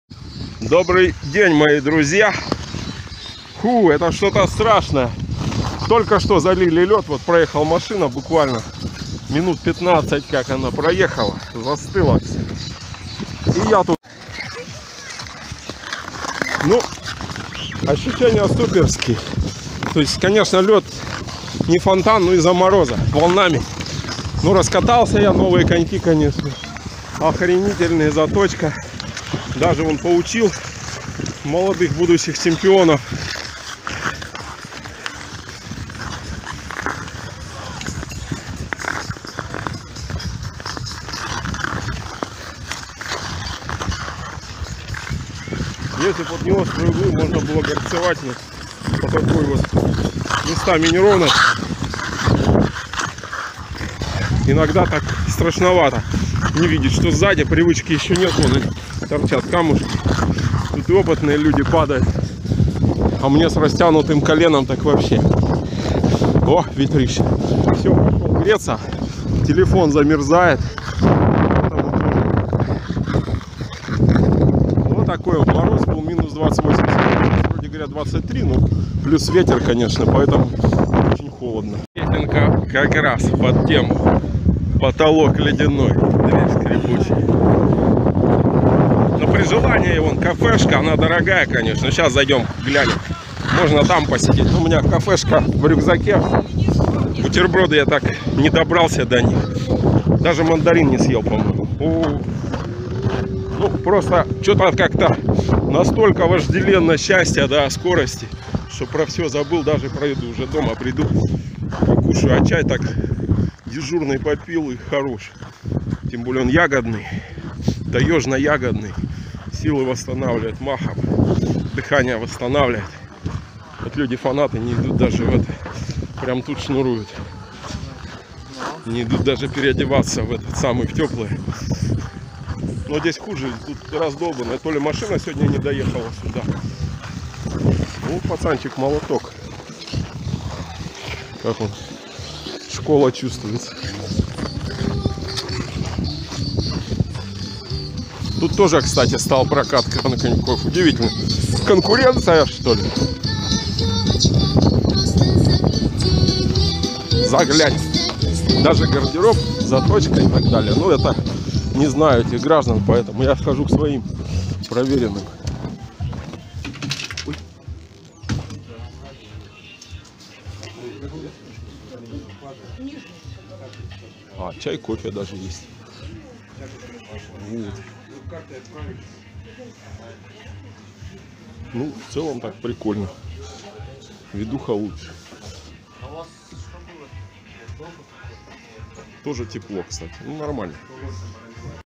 Комсомольск - открытый каток у берега Амура, на Речном Вокзале
мороз прыгал от минус 28 с утра до минус 22 в обед, но холодный ветерок с Амура жжёт лицо и заставляет не сидеть. Тестирую только, что приобретенные новые коньки на свежем льду 2020 !